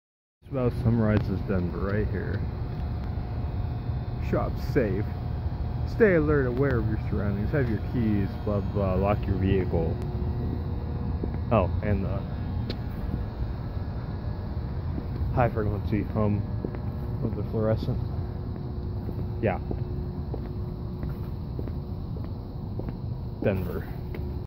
Denver
Out of the mountains down to the overpopulated great plains today. Not as bad as public restrooms on the West coast having a used heroin needle collection boxes but when they have to put signs up that you need to be aware and lock your vehicle something ain't right.  One thing comical about the sign is it says not to leave valuables in but to lock the doors, that's a recipe for a broken window, a lose-lose for both the scumbag and owner of the vehicle.

Able bodied man

Headband from Julia at CaliforniaPatchwork.Etsy.com